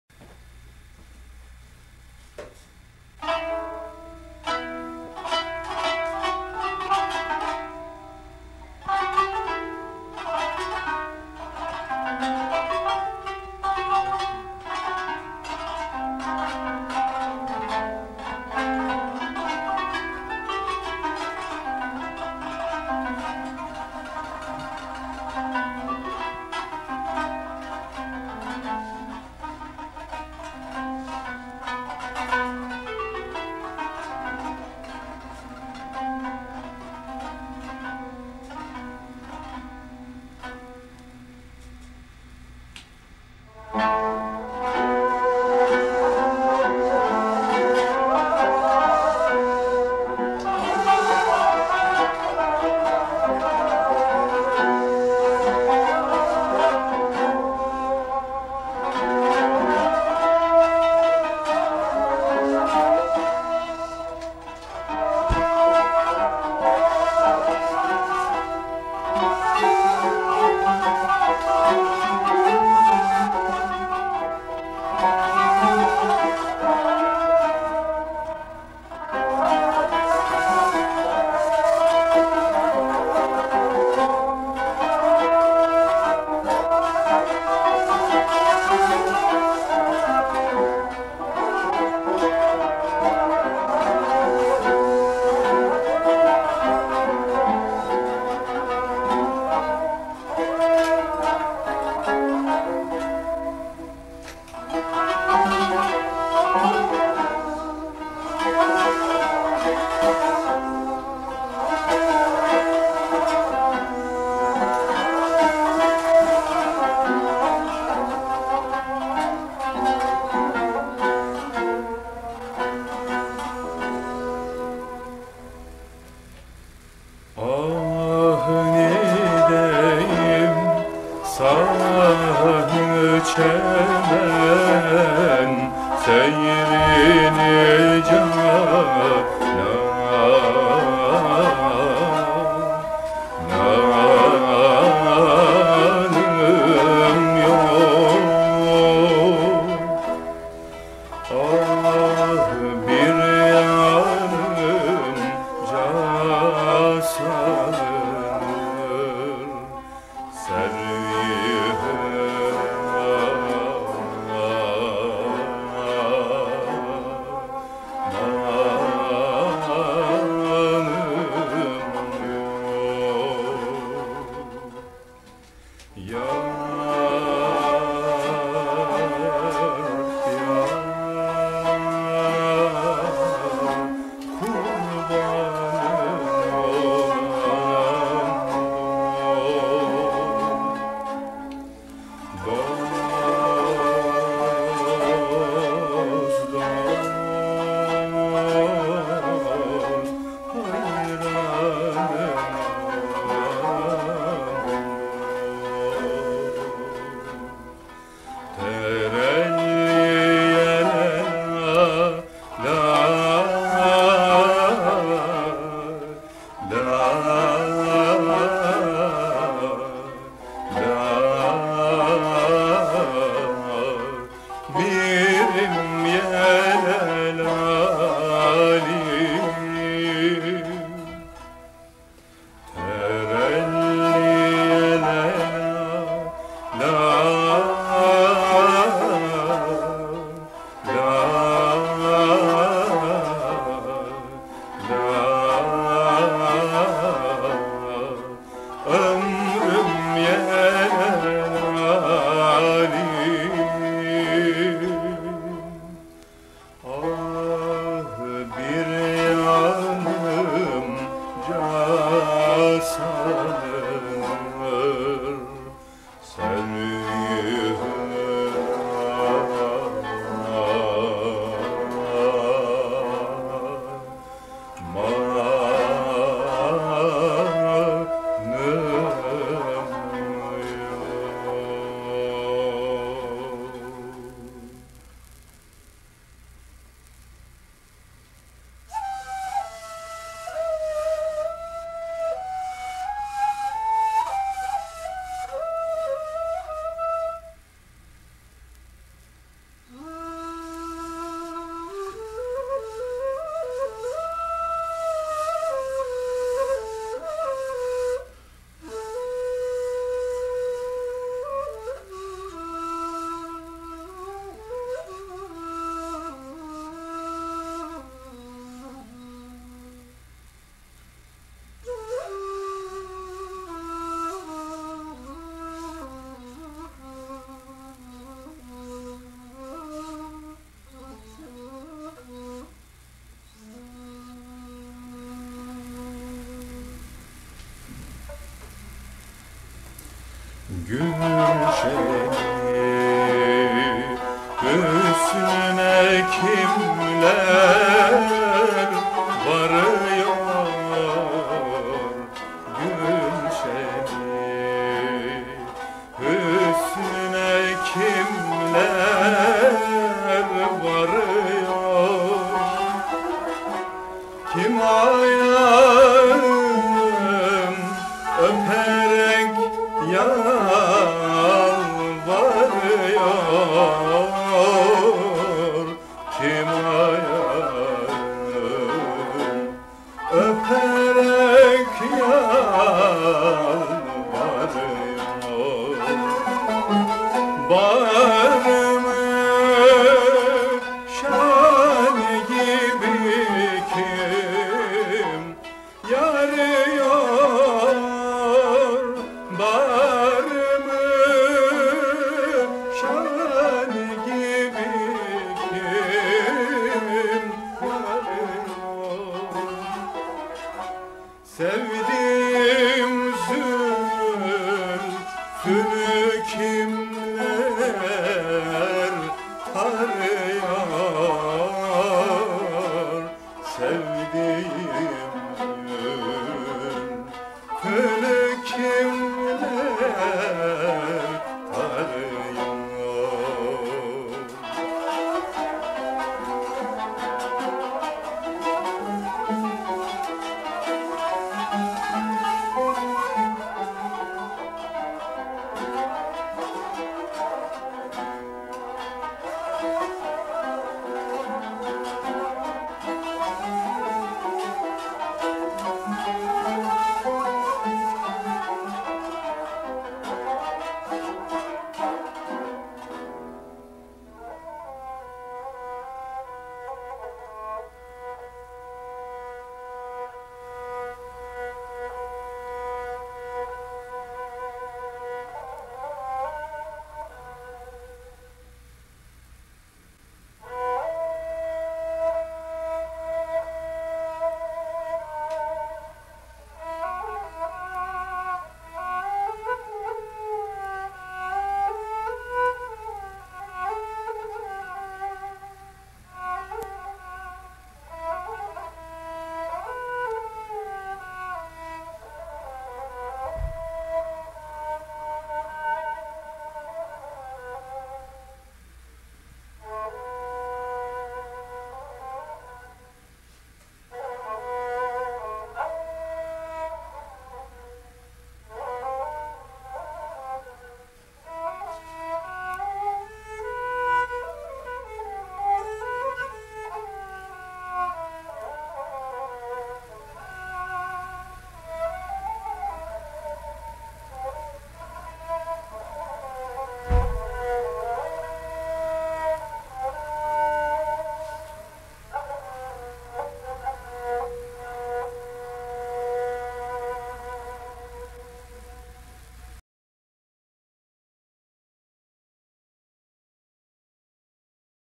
Turkish Ottoman Classical Music Ensemble - Hicaz Fasi
Robert Garfias Archive

Filmed in 1972. Kanun: Cuneyd Kosal, Vocal: Birgit Recep, Ney: Dogan Ergin, Kemence: Cuneyd Orhan, Tanbur: Necdet Yasar.